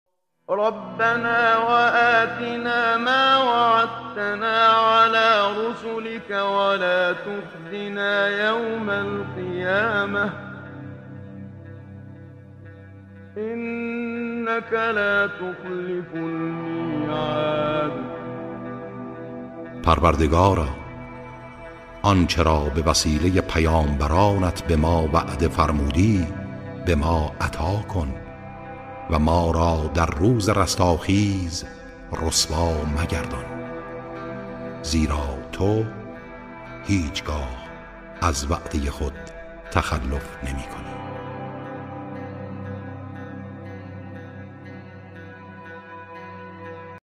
هر روز یک آیه قرآن کریم
پروردگارا# آنچه را به وسیله پیامبرانت به ما وعده فرمودی به ما عطا کن و ما را در روز رستاخیز رسوا مگردان یرا تو هیچگاه از وعده خود تخلف نمی‌کنی آل عمران 194